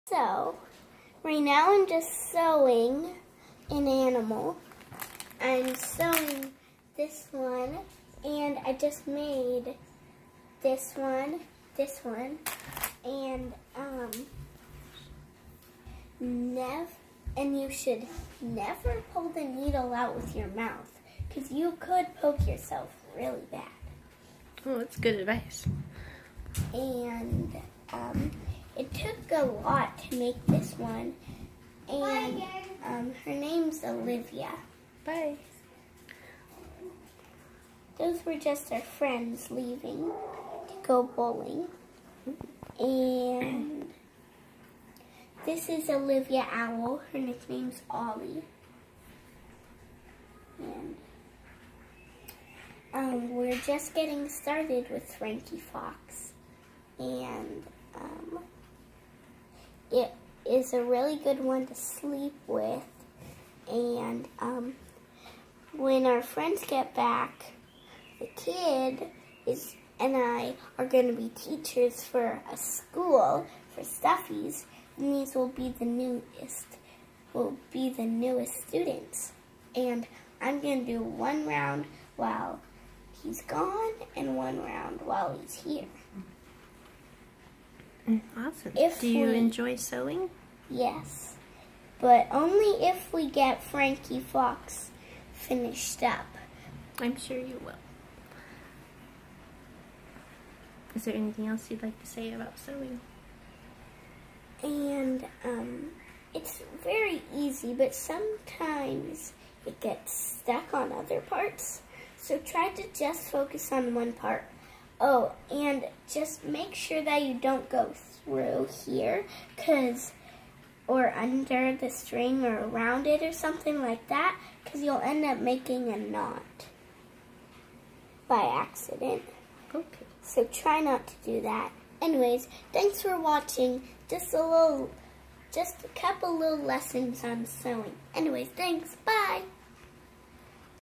Eleven gives sewing lesson for kids, felt animal creation, fox and owl
Sewing for children